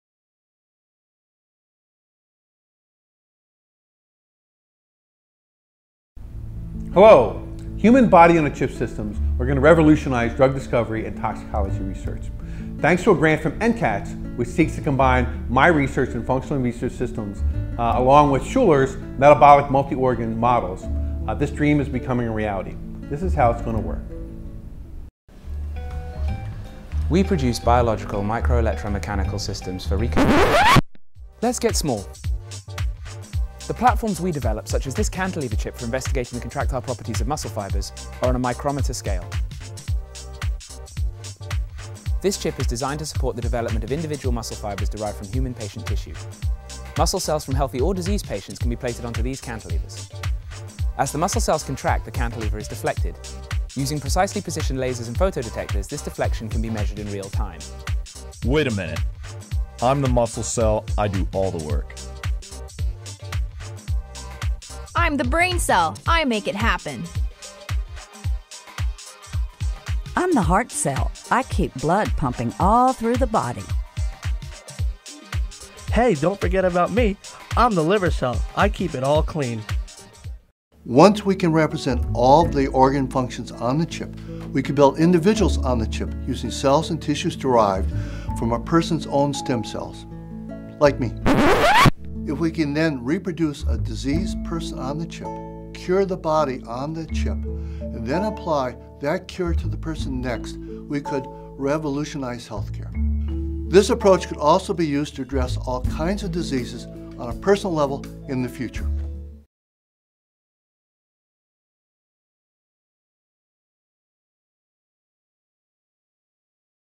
Body in a Chip
We are funded to produce biological microelectromechanical systems which recapitulate human tissue functionality. Such technology focuses on cultures of human cells within novel platforms that allow for assessment of physiologically correct functional outputs. Assays include measurement of electrical activity in neurons contractile activity in muscle and enzyme production in liver cells. By integrating such systems with microfluidic housings we aim to accurately model absorption distribution metabolism and elimination as well as toxicology of drugs in a human test system. The successful development of this model will accelerate drug trials and provide more predictive data pertaining to compound performance in humans.